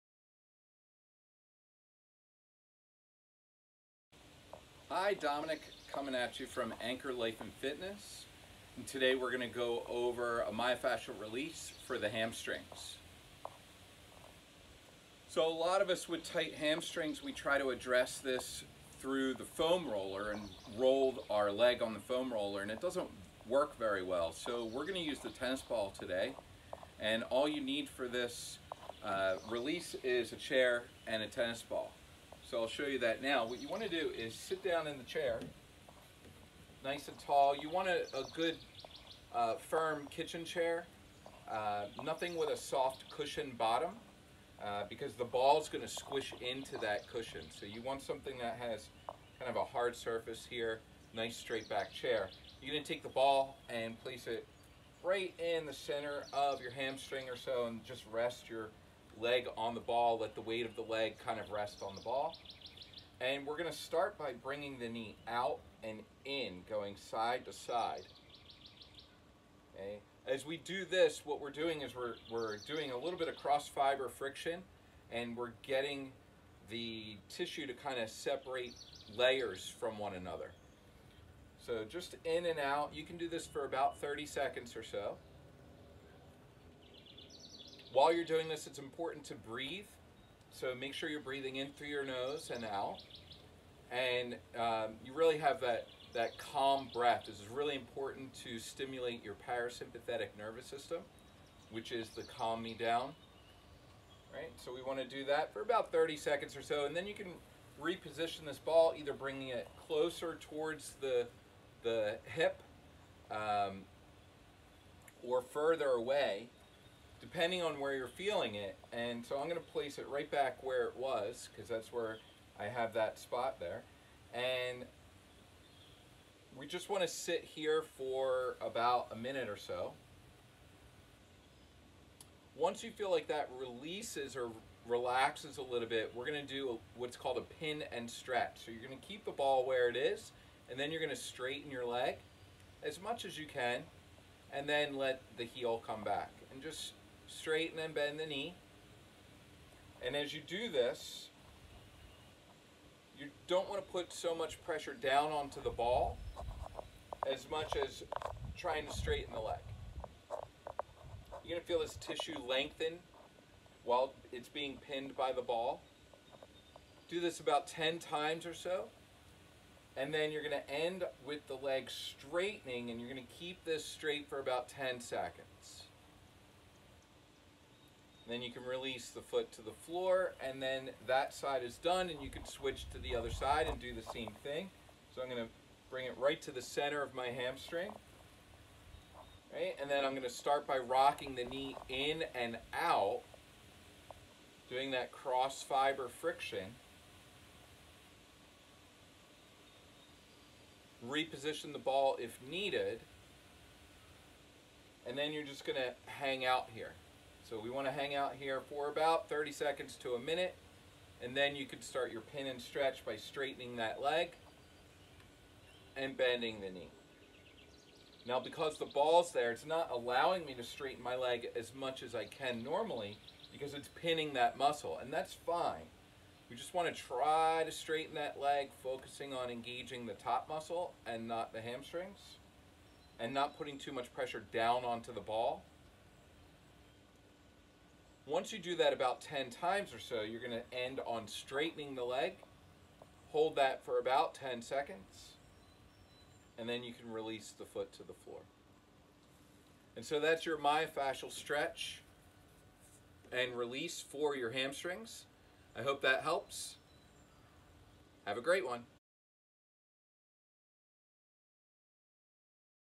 Hamstring Myofascial Release
Have rocks for Hamstrings?  Here is a simple way to help release some of the tension!